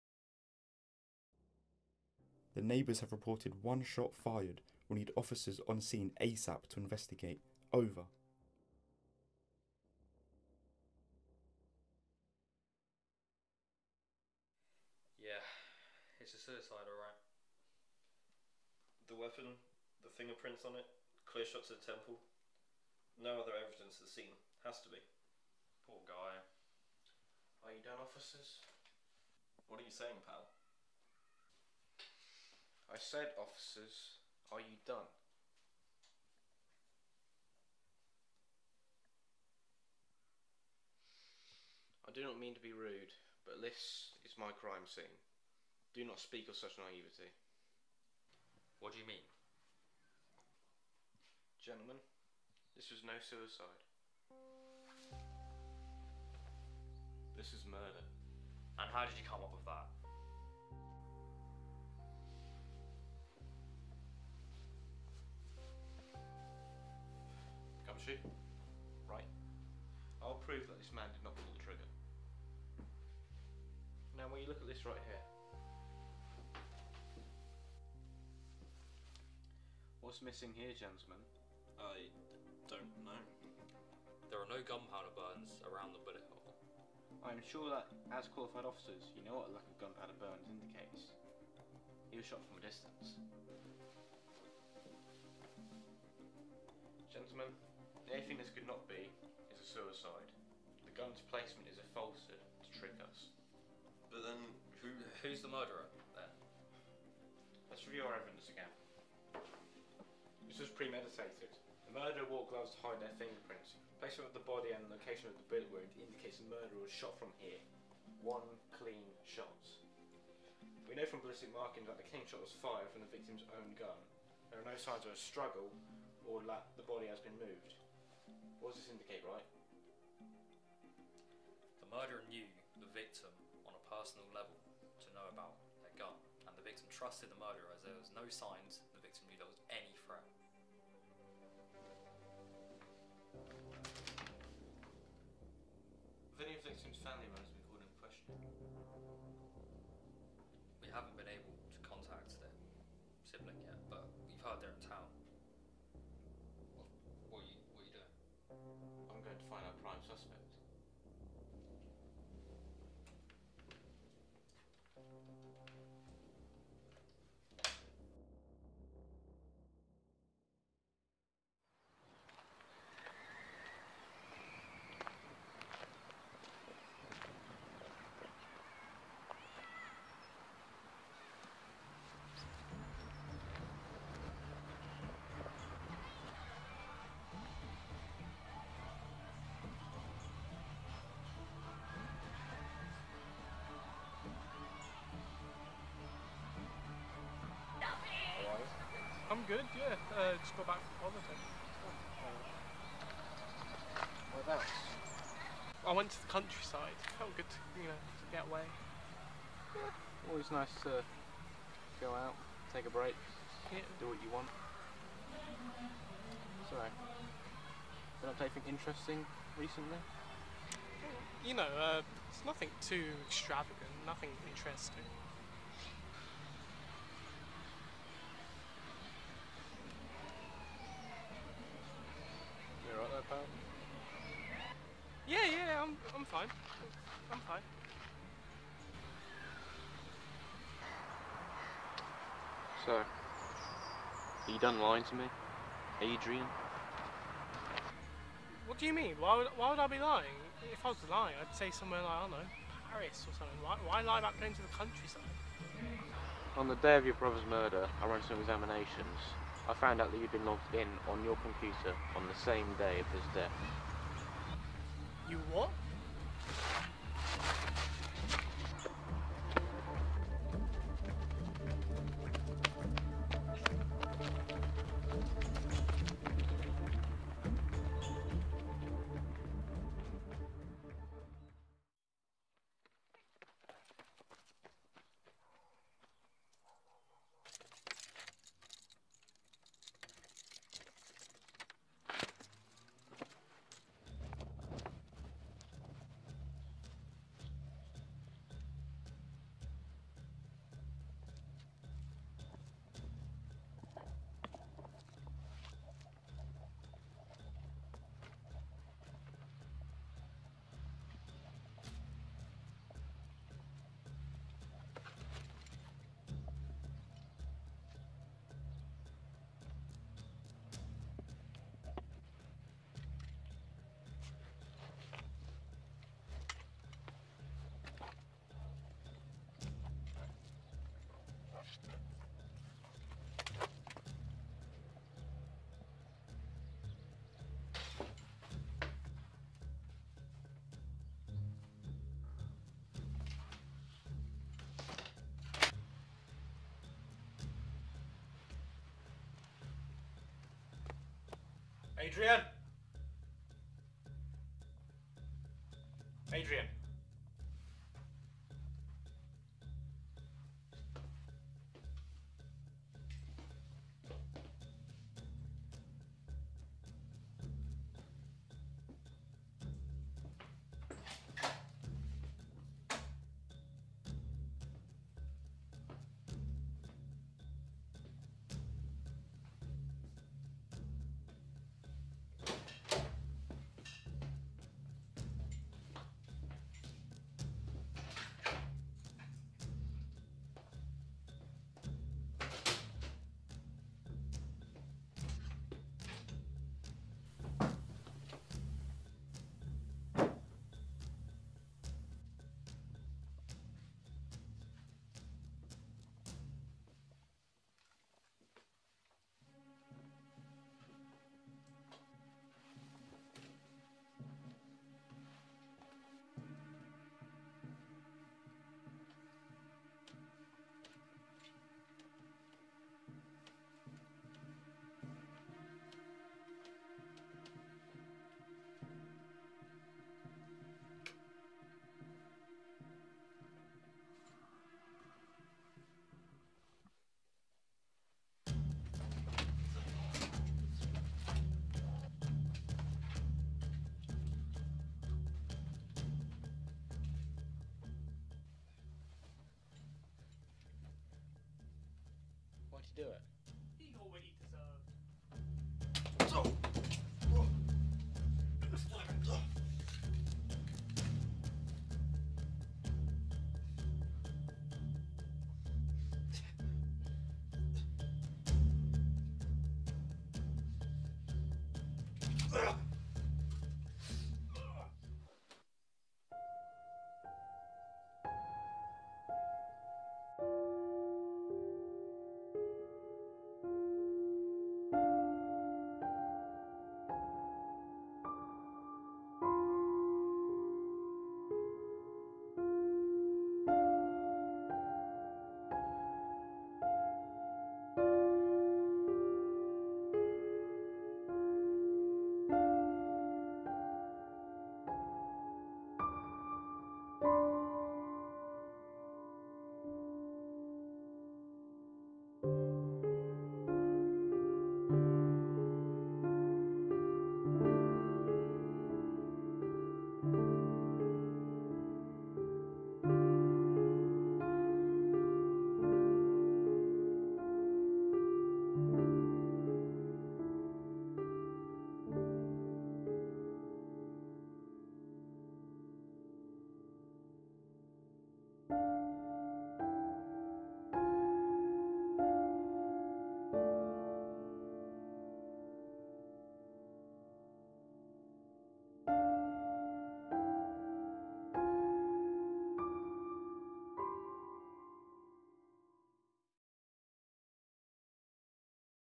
One Clean Shot - Student Short Film (Full Version)
After 5 months of work, including many hours of filming across 4 days and way more than 15 hours of editing over a few weeks, as well as weeks of scripting, storyboarding and prep work, the full version is finally here! Obviously it is riddled with flaws, but hopefully there is something you can appreciate here.

This was completed as coursework for my B-Tec Creative Media course. 00:00 to 2:50 was directed/filmed/edited/scripted/storyboarded by me. 2:51 to 4:41 was directed/filmed/edited/scripted/storyboarded by Ben Kirwin. 4:42 to 8:00 was directed/filmed/edited/scripted/storyboarded by Ethan Bailey.

You can also find my Director's Commentary for my section in the playlist linked in the cards.

Thank you so much to all the cast for making this possible.